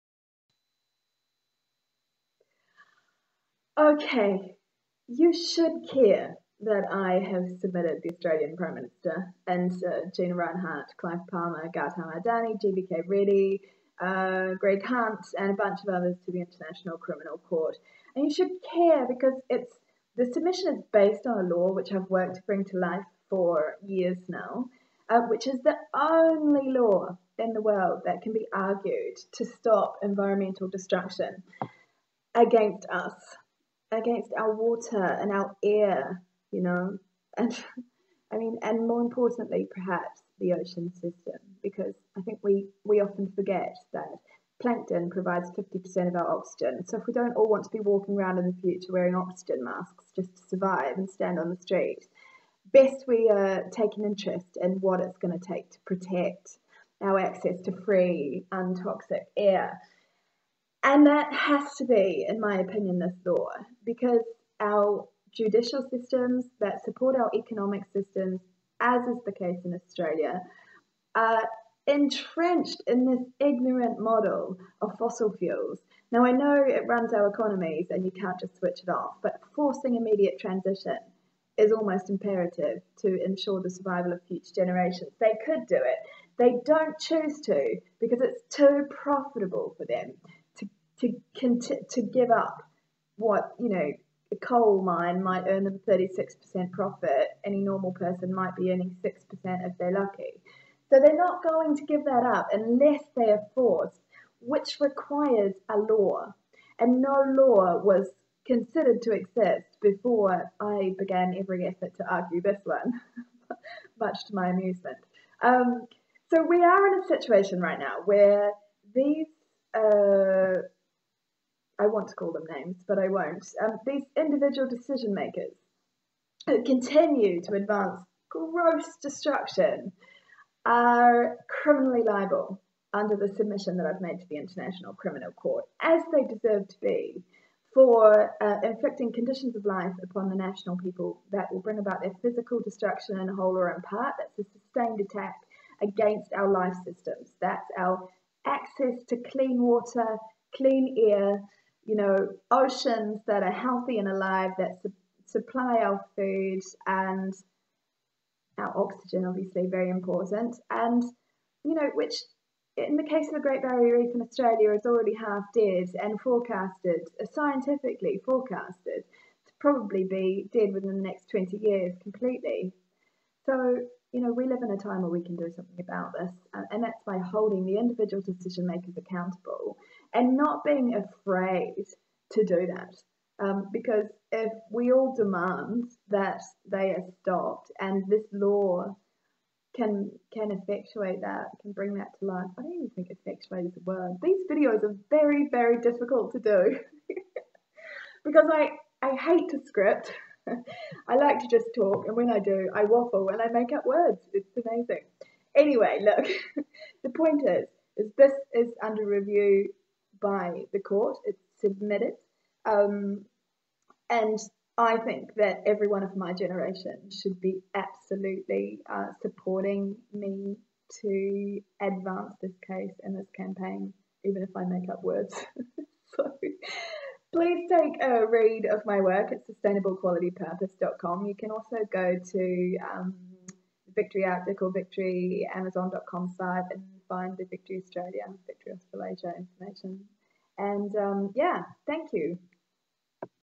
Love it.
Love it.  I say 'these guys are killing you slowly, destroying your children's health and future but we can stop them', and i get conversations about my lipstick and cleavage in response?  Really not comparable with the importance of protecting your oxygen supply, clean water, fresh air, safe food, liveable temperatures, coastal cities not being sunk, a chance to reverse the cycle causing the floods, fires, heat waves and so much premature death from illness and disease. So thank you for your very important feedback about that which has nothing to do with my point of bringing this existing law to life that protects our oxygen, food, water, marine life, air and temperature.   Here is another attempt to be less legal and explain better (until sometime between 3 and 4 minutes, lol).  I know, still not perfect but one day, a good concise video is coming.